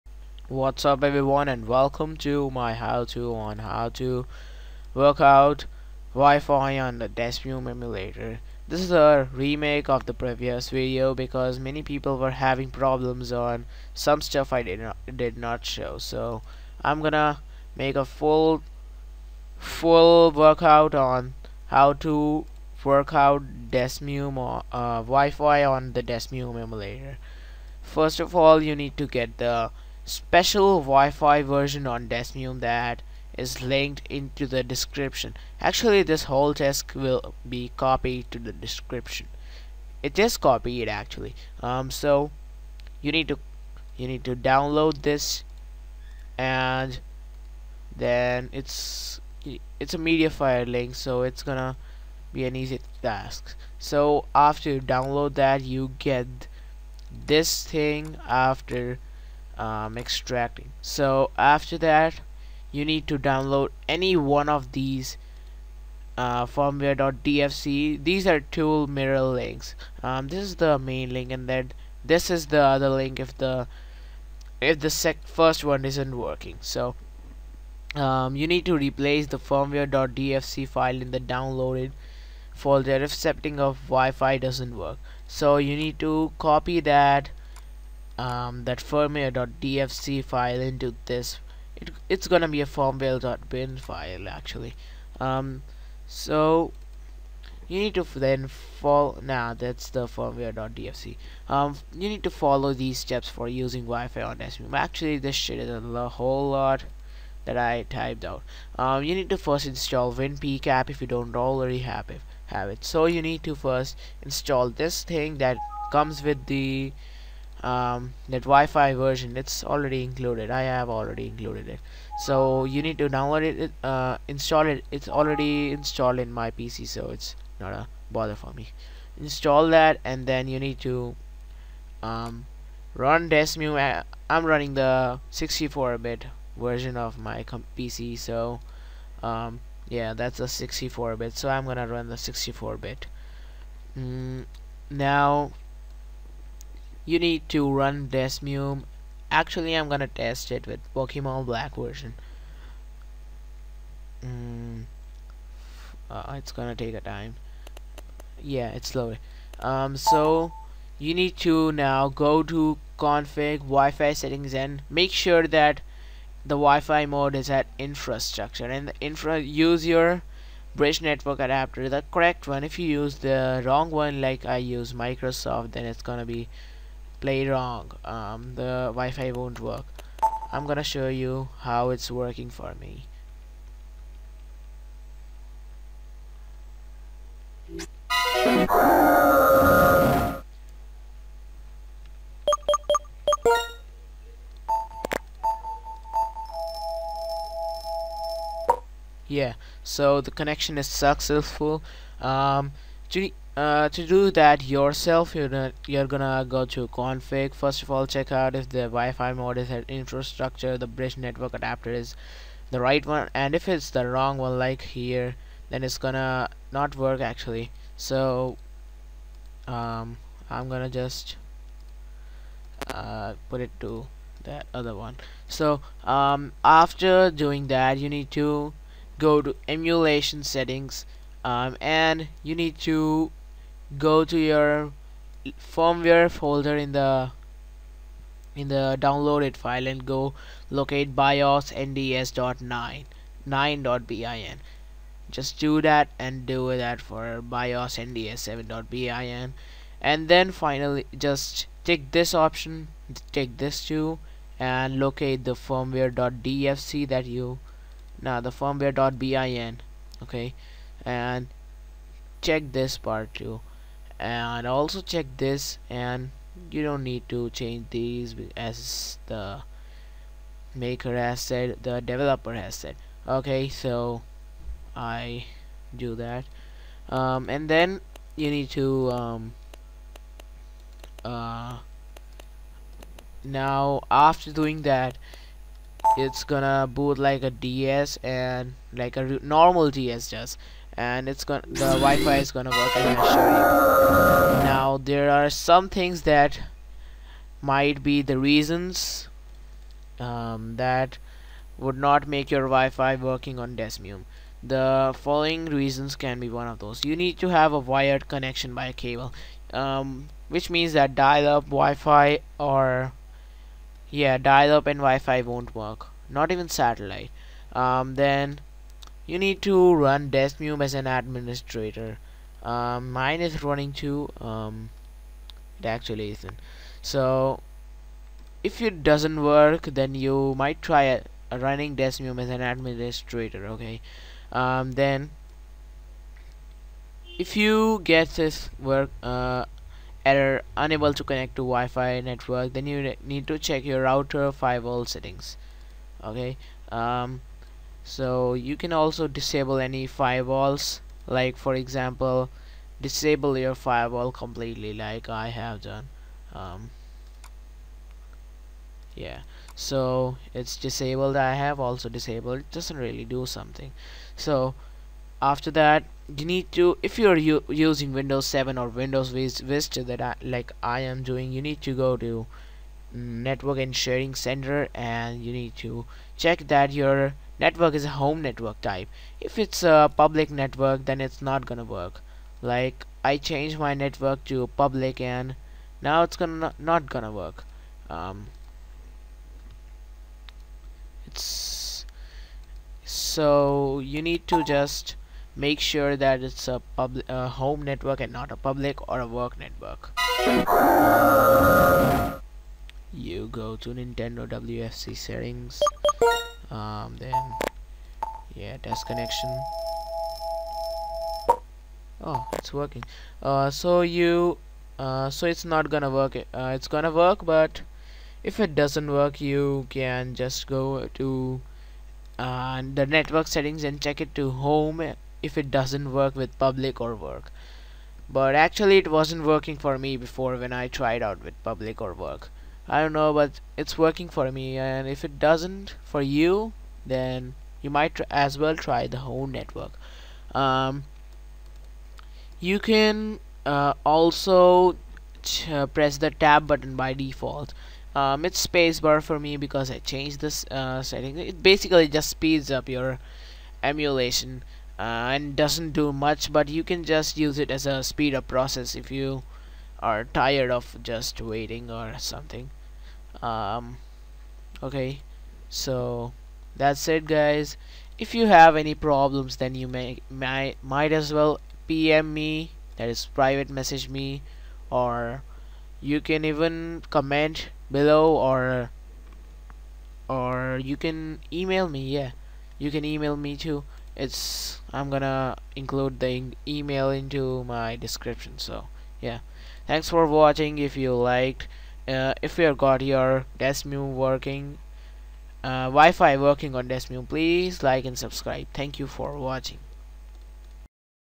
How to use Wi-Fi on DeSmuMe
*Replace the firmware.dfc fle in the downloaded folder, If setting up Wifi doesnt work.

Follow these steps for using Wifi on desmume:

1. Install WinPCap if you do not already have it. It comes in the folder you download.
2. Run DeSmuME and go to Config then Wifi Settings.
3. Change to infrastructure and use your network card that your computer uses for internet.
4. Go to Config then emulation settings.
5. Check the box "Use external BIOS images".
6. Put biosnds9.bin that is in the Wi-Fi folder in "ARM9 BIOS image".
7. Put biosnds7.bin that is in the Wi-Fi folder in "ARM7 BIOS image".
8. Check the box "Use external firmware images".
9. Put the firmware.bin in "Firmware image"
10. Check the box "Boot from firmware".
*This makes your emulator run like the DS with the home screen and etc. It is not necessary to have this for it to work but the first time you are doing this just check it to be safe.
11. Go to "Nintendo Wi-Fi connection settings"/"Nintendo WFC setup" with any game with wifi capabilities.
12. Test the Wi-fi to make sure you see SoftAP on Connection 1 and also that it connects. You might have to test it a couple of times but don't do it more than 10.

Conditions for Using Wi-fi:

- Have a wired connection by a cable (lan cable)
-Run DeSmuME as an administrator
-"For anyone getting 'Unable to get IP address' or 'Unable to connect to Nintendo Wi-fi network' errors, you need to check your router firewall settings. I use DD-WRT and an enabled SPI Firewall will not allow you access, disabling that let everything work fine.
-Disabling any firewalls that you think would be blocking the process...
-WINDOWS 7/WINDOWS VISTA users must go to network and sharing center and make sure that your connection type is set to HOME network and NOT PUBLIC OR WORK(Work might work but just try Home)
-In the Wi-Fi settings in DeSmuMe make sure it is set to infrastructure mode and also the correct bridging is enabled with your ETHERNET CONTROLLER (your network adapter)
- Try pressing tab when connecting to Nintendo Wi-Fi connection, for faster speed.

If there are any errors connecting to the NWFC, just see the error code causing this, and go to the website, find your matching error code, and see the error: